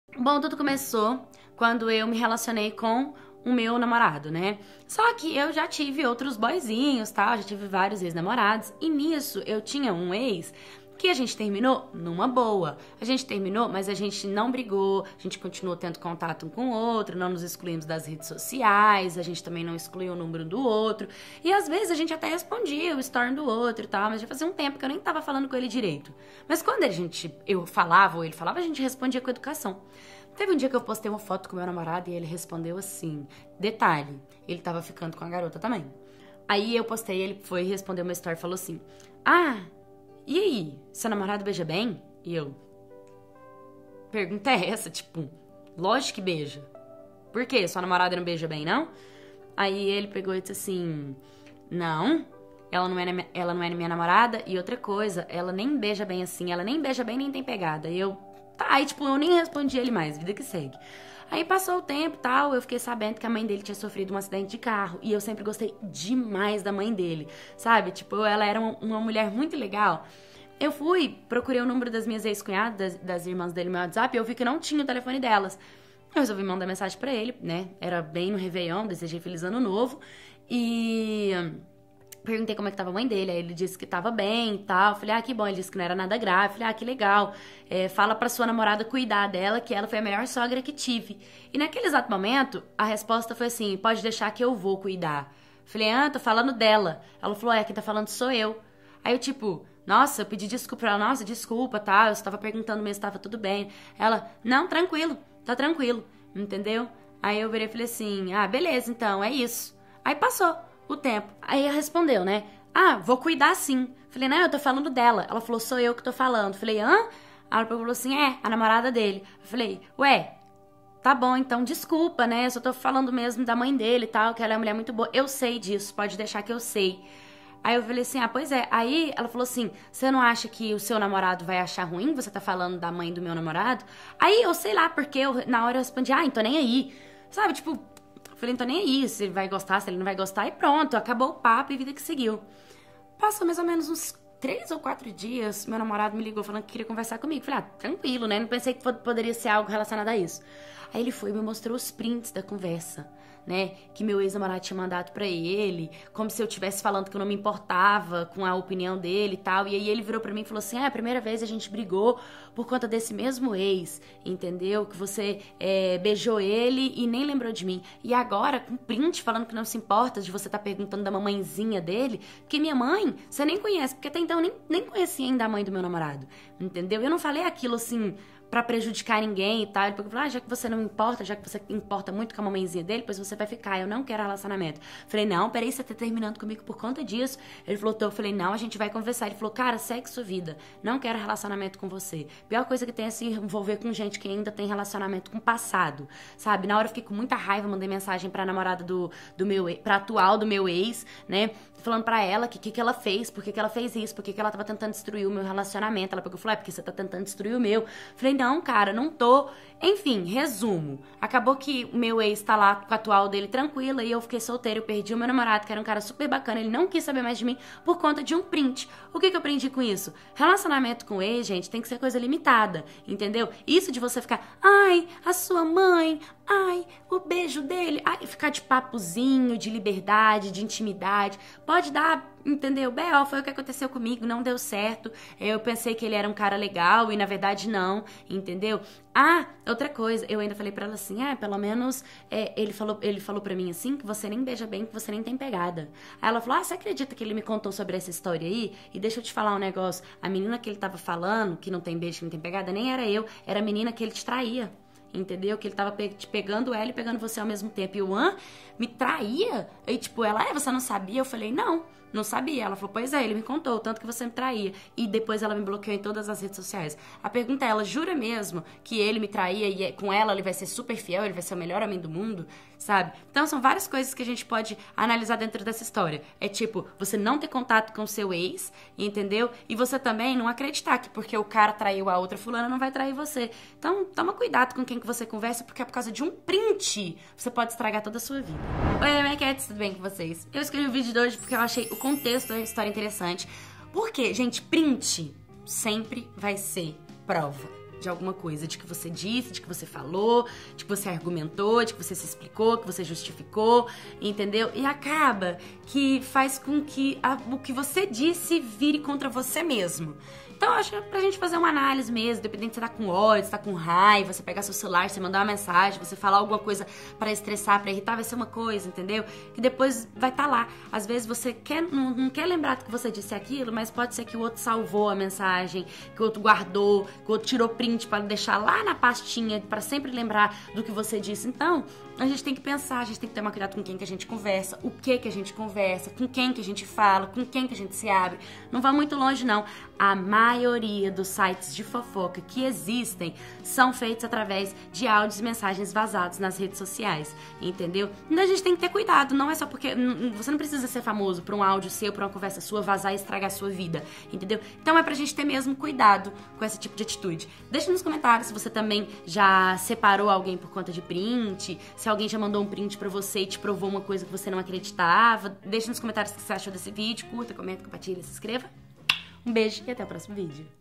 MEU EX MANDOU PRINT
Oi oi oi my cats, tudo bem com vocês? A história de hoje é sobre uma menina que tinha um ex namorado que acabou virando seu amigo, pelo menos era o que ela achava. Até que eles começaram a conversar sobre assuntos mais delicados e íntimos, mas sem segundas intenções. A nova namorada dele viu essa conversa e não gostou nada do papo e começou a responder e pedir que ela não falasse mais nada. A menina resolveu seguir sua vida, mas não esperava que a atual do seu ex fosse mandar os print da conversa pro seu namoro e tentar acabar com seu relacionamento. De uma conversa inocente pra uma grande confusão e término de namoro. Quem vocês acham que terminou? Vejam o final dessa história e me contem: vocês têm uma boa relação com ex namorado (a) e acham saudável ter uma amizade com ele/ela depois de terminar o namoro?